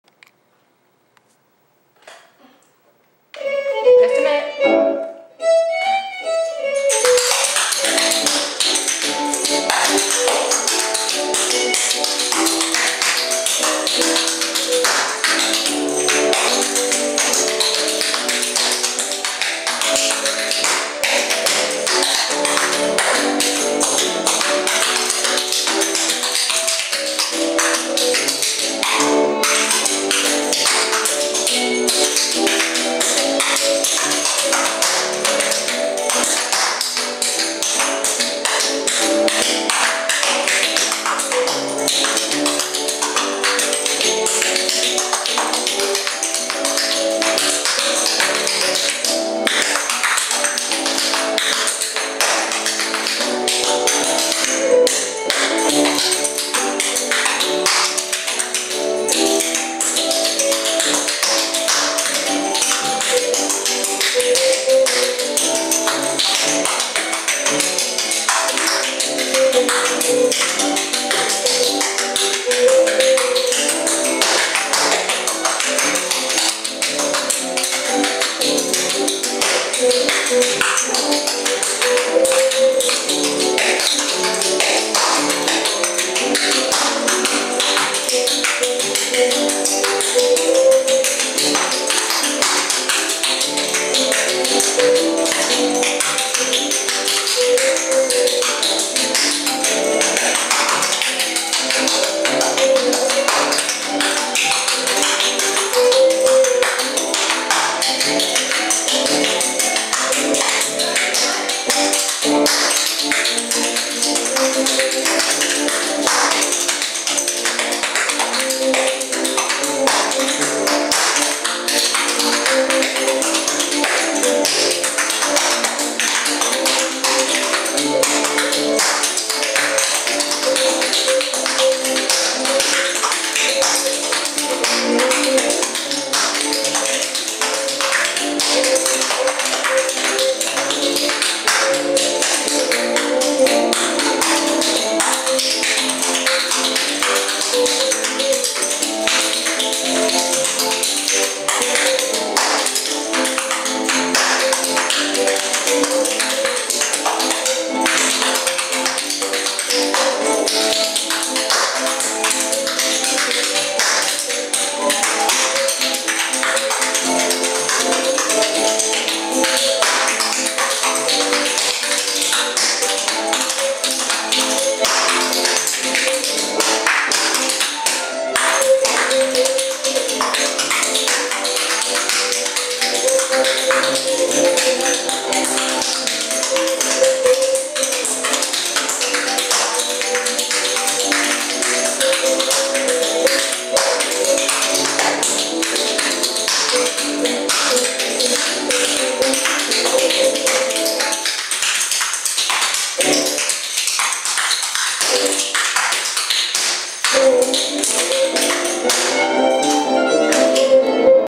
Sarajayne practicing 2009
Clog, jig, reel
Practice at Linda's
June 26,2009